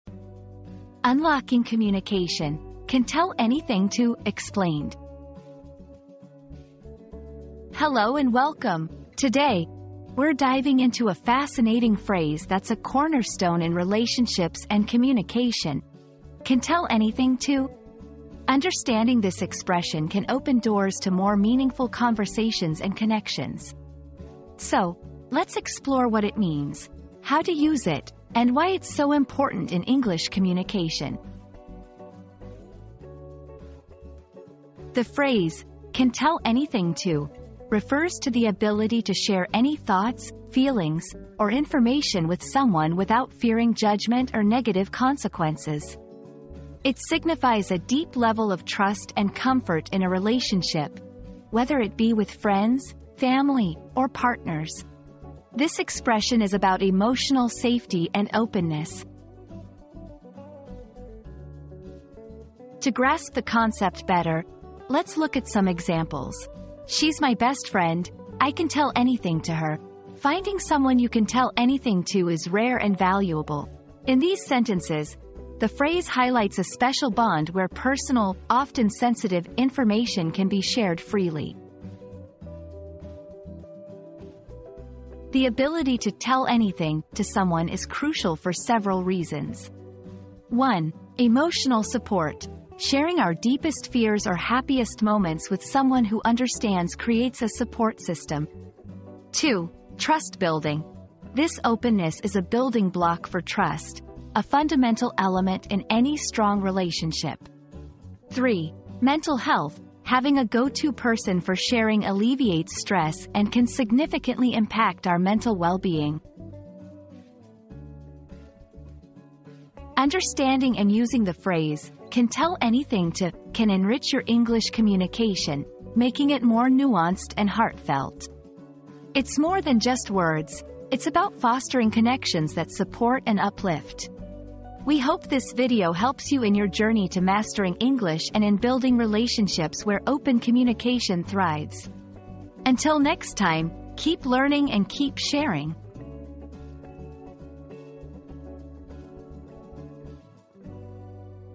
Unlocking Communication: "Can Tell Anything To" Explained
Unlocking Communication: The Power of 'Can Tell Anything To' • Discover the key to effective communication and building trust with the 'Can Tell Anything To' approach. Learn how this simple concept can transform your relationships and conversations.

00:00 • Introduction - Unlocking Communication: "Can Tell Anything To" Explained
00:34 • Understanding the Phrase
01:02 • Usage in Sentences
01:24 • Why It Matters